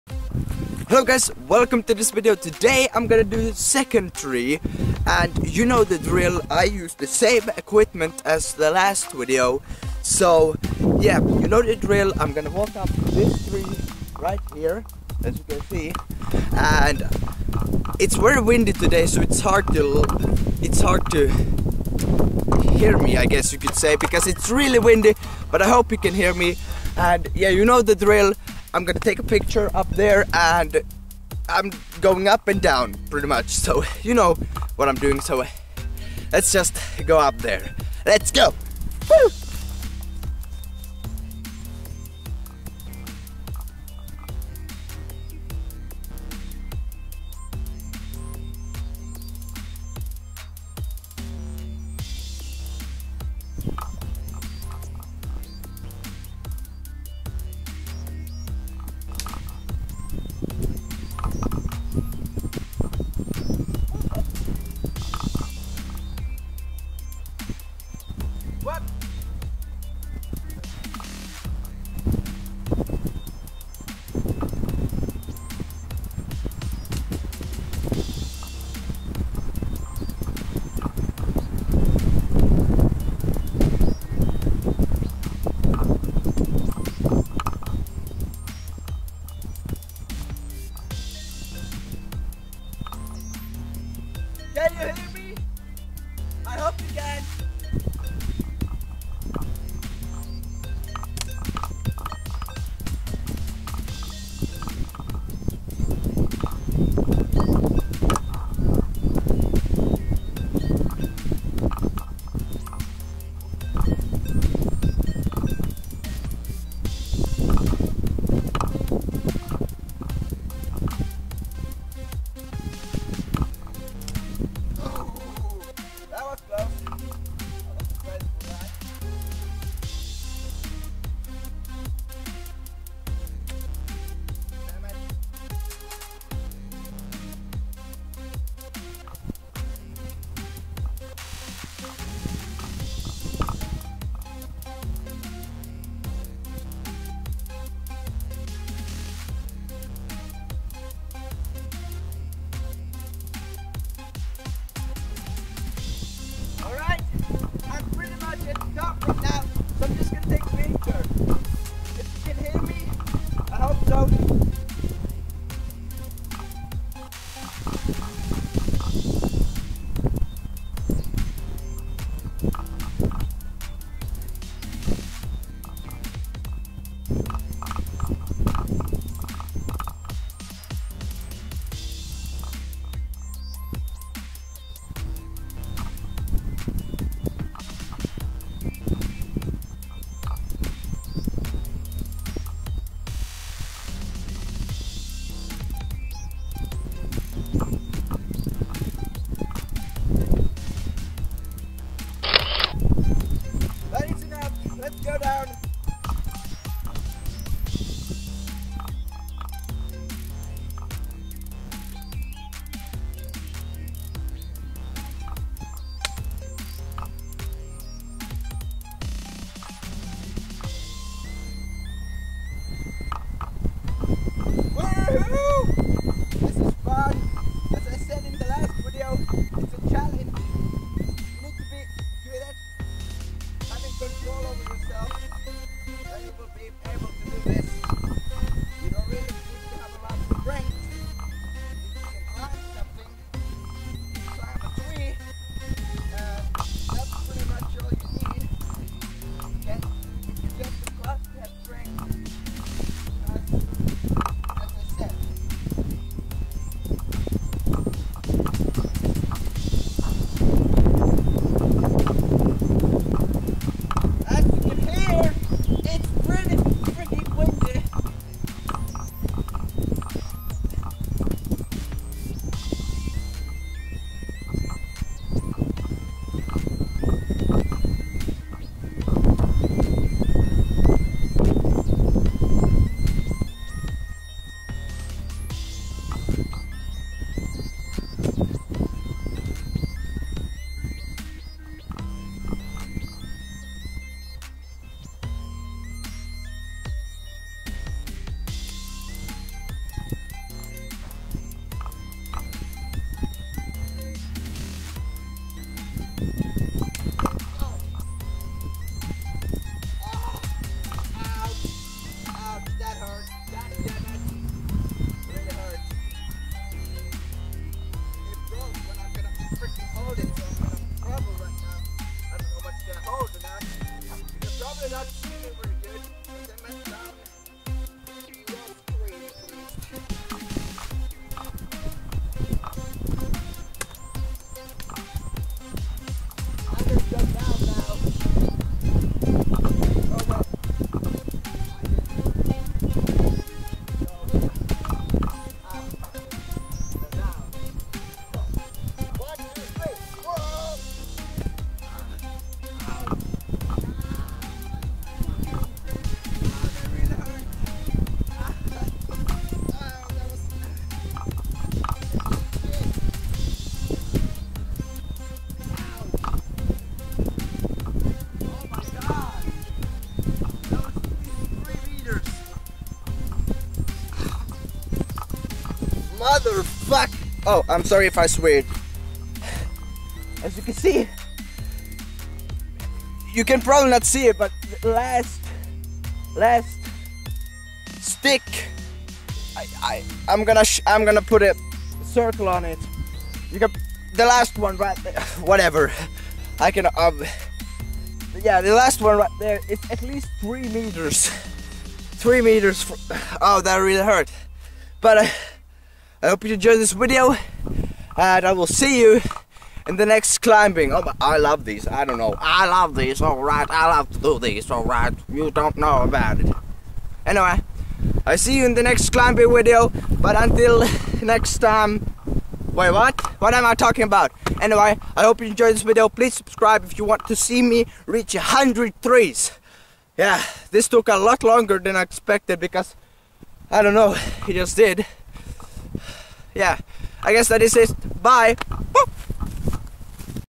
Tree Climbing - 2nd Tree
Today I will be climbing my 2nd tree

(Rules: No Shoes, No Ropes)
(My goal is to climb up 100 trees without any damage.)

(Twitter Account Coming Soon)